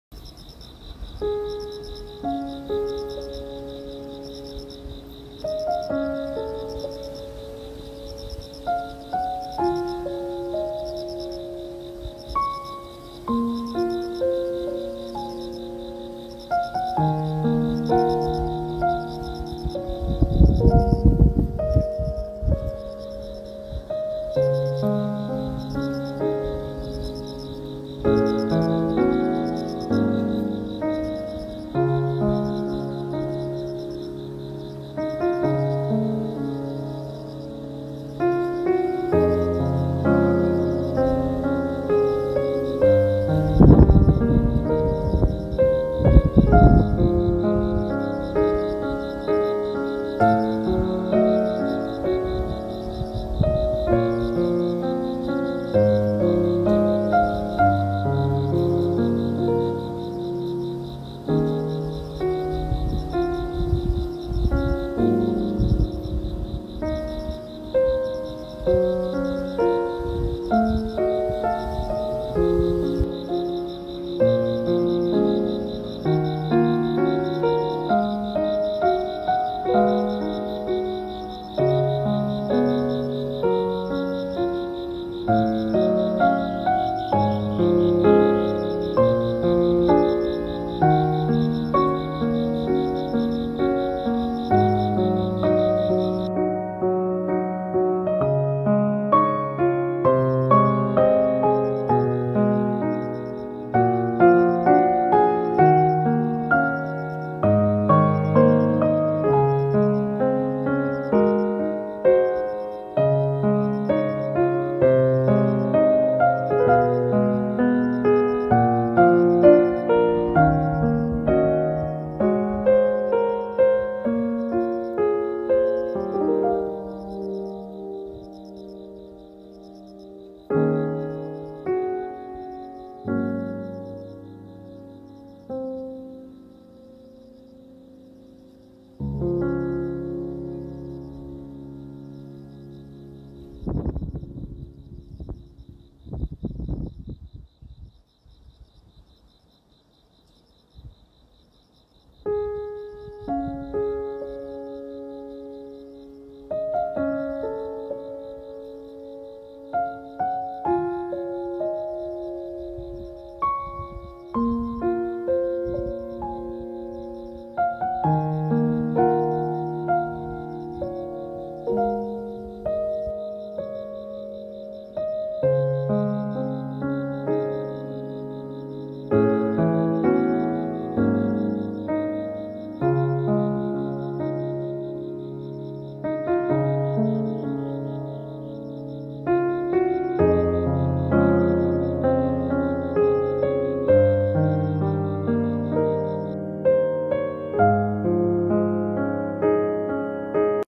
구름에 달가는 중... 가을이 오는 소리...
구름에 달이 가고, 귀뚜라미 소리가 가을을 부릅니다.